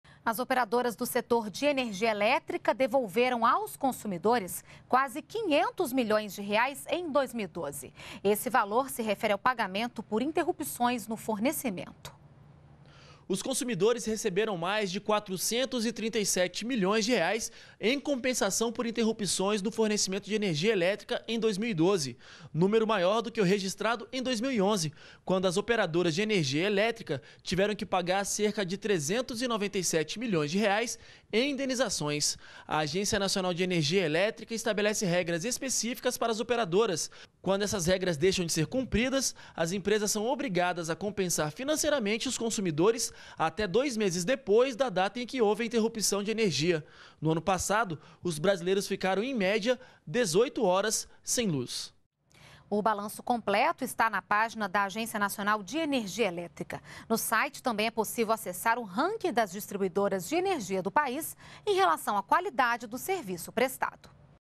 Falta de luz faz operadoras devolverem quase R$ 500 milhões a consumidores em 2012
NBR NOTÍCIAS - 29.03.13: As operadoras do setor de energia elétrica devolveram aos consumidores R$ 437 milhões no ano passado. O valor, relativo a indenizações por interrupções no fornecimento do serviço, é superior ao pago em 2011, quando a falta de luz custou R$ 397 milhões às operadoras. A medida é fiscalizada pela Agência Nacional de Energia Elétrica (Aneel), que estabelece regras para as empresas. Em 2012, os brasileiros ficaram, em média, 18 horas sem energia elétrica.